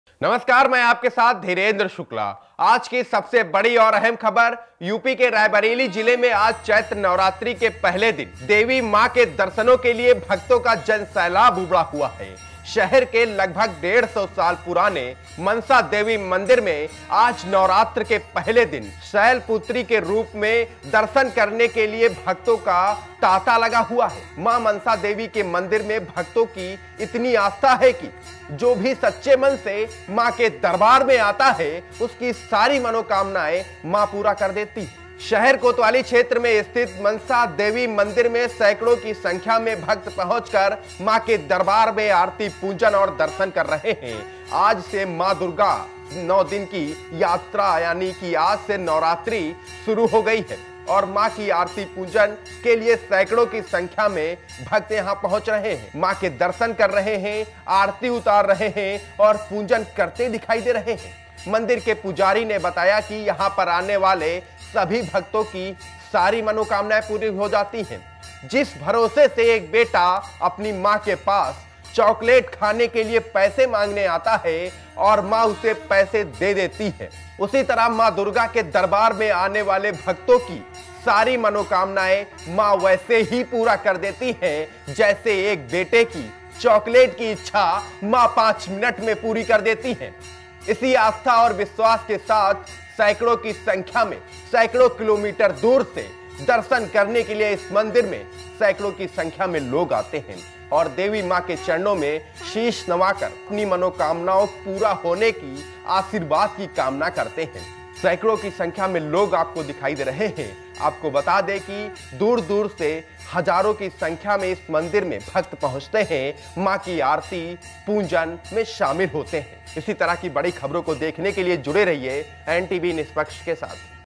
रायबरेली मनसा देवी मंदिर में हजारों की संख्या में भक्तों ने पहुंचकर की मां अंबे की पूजा
Edit by : Puneet Kumar